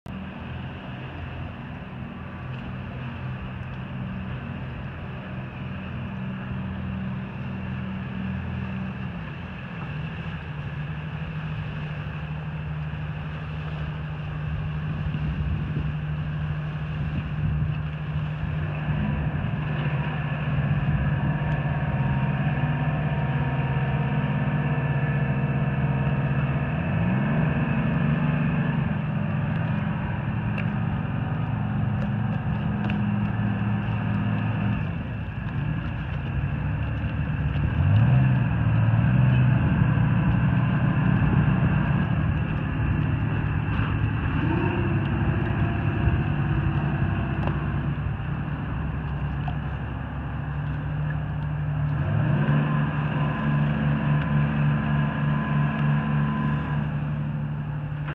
Fire boat for sale - Celtic 3 - Pacific Boat Brokers
Aluminum 40 fire boat built in 1992 by Celtic Shipyards. Powered by twin 6-71 turbo 450 hp each diesels with Hamilton 291 jets. Cruise speed at 30 knots.  Ideal for forestry, industrial and municipal fire response. This quality build is fully rigged and ready to work.